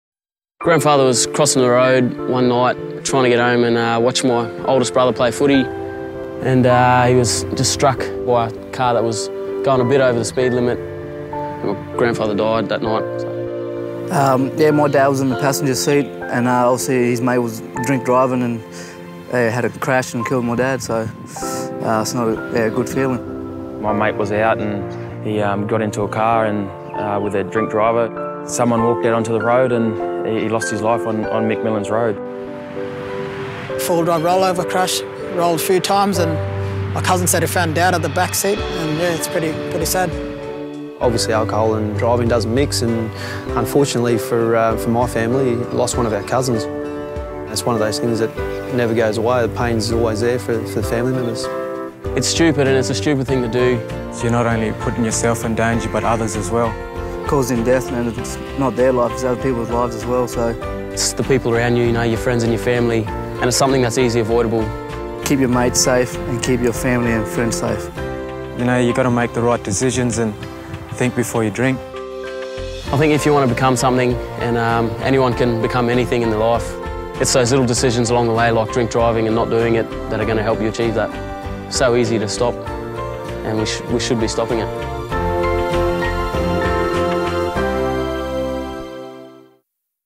Radd TVC - AFL Stars speak out
Drawing on their personal stories, Alwyn Davey, Andy McLeod, Austin Wonaeamirri, Charlie Maher, Daniel Motlop and Steven Motlop explore the devastating and destructive impact of drink driving on the lives of loved ones and the long-term effects on communities. The players draw attention to the need for individuals to be responsible, keep friends and family safe, and ensure their own safety from something so easily avoidable.